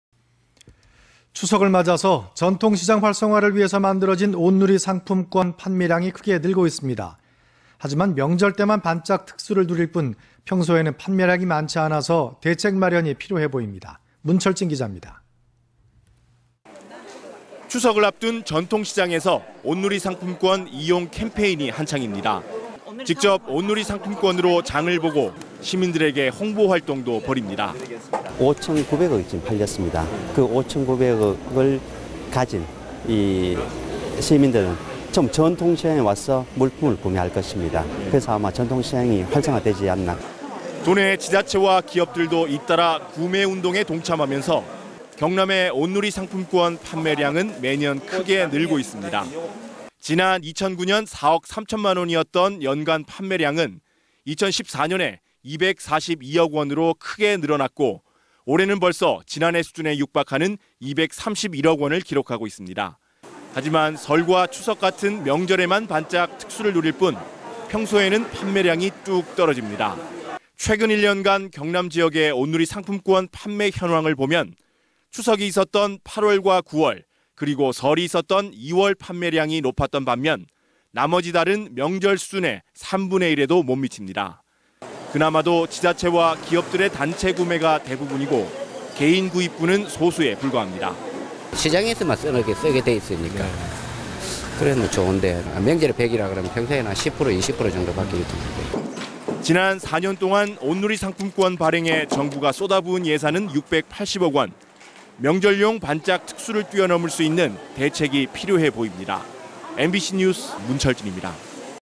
MBC경남 뉴스데스크 2015 09 22 온누리 상품권 명절에만 '반짝 특수'
추석을 맞아 전통시장 활성화를 위해 만들어진
온누리 상품권 판매량이 크게 늘고 있습니다.

하지만 명절에만 반짝 특수를 누릴 뿐
평소에는 판매량이 많지 않아
대책 마련이 필요해 보입니다.

문철진 기자!

추석을 앞둔 전통시장에서
온누리상품권 이용 캠페인이 한창입니다.

직접 온누리상품권으로 장을 보고
시민들에게 홍보활동도 벌입니다.

엄진엽/경남중소기업청장
"(전국에서 올해 온누리상품권)
5천 900억 원어치가 팔렸습니다.
그 5천 900억을 가진 시민들이 전부 전통시장에
와서 물품을 구매할 것입니다. 그래서 아마
전통시장이 활성화되지 않을까... "

도내 지자체와 기업들도 잇따라
구매 운동에 동참하면서
경남의 온누리상품권 판매량은
매년 크게 늘고 있습니다.

지난 2009년 4억 3천만 원이었던
연간 판매량은 2014년에
242억 원으로 증가했고
올해는 벌써 지난해 수준에 육박하는
231억 원을 기록하고 있습니다.

하지만 설과 추석 같은 명절에만
반짝 특수를 누릴 뿐
평소에는 판매량이 뚝 떨어집니다.

최근 1년 간 경남지역의
온누리상품권 판매 현황을 보면
추석이 있었던 8월과 9월
그리고 설이 있었던 2월 판매량이
높았던 반면 나머지 달은
명절 수준의 3분의 1에도 못 미칩니다.

그나마도 지자체와 기업들의 단체 구매가
대부분이고 개인 구입분은 소수에 불과합니다.

이영성/시장 상인
"시장에서만 쓰게 되어 있으니까 많이 사면
좋은데...명절이 100이라 그러면 평소에는
10~20% 정도 밖에 유통이 안 돼요."

지난 4년 동안 온누리상품권 발행에
정부가 쏟아 부은 예산은 680억 원,

명절용 반짝 특수를 뛰어 넘을 수 있는
대책이 필요해 보입니다.

MBC NEWS 문철진